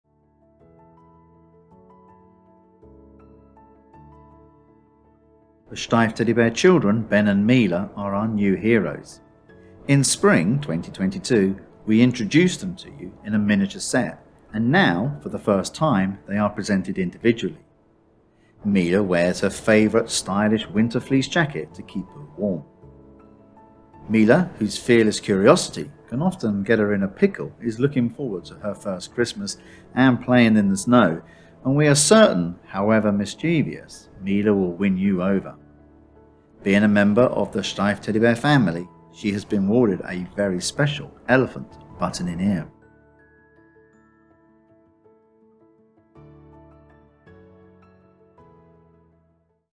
Steiff's Mila Teddy Bear with Winter Jacket EAN 007224
The Steiff Teddy Bear children Ben and Mila are our new heroes. In spring we introduced them in a miniature set, and now for the first time they are presented individually. Mila Teddy Bear wears her favorite stylish winter fleece jacket to keep her warm. Mila whose fearless curiosity can often get her in a pickle is looking forward to her first Christmas and playing in the snow. We are certain, however mischievous, Mila will win you over. Being a member of the Steiff Teddy bear family she has been awarded a very special "Button-in-Ear" with an elephant motif!